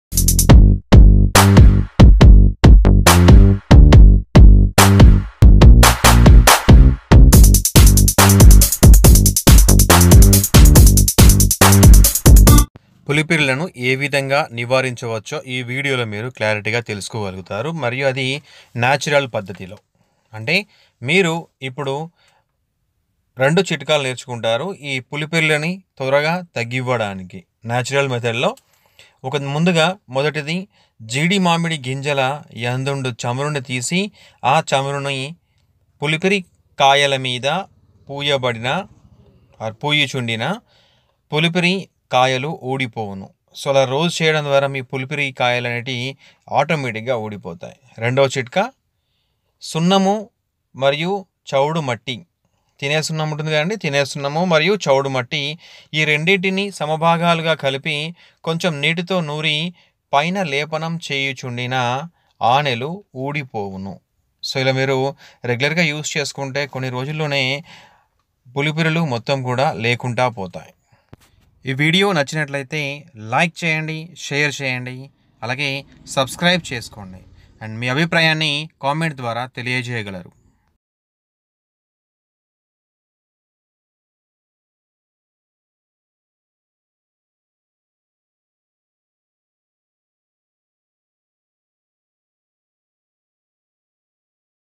పులిపిరిలను తీసివేయడం ! Pulipiri remove ! RS Creative Videos ! HEALTH TIPS
How to remove pulipiri in telugu in natural method without side effect by RS Creative Health Tips in telugu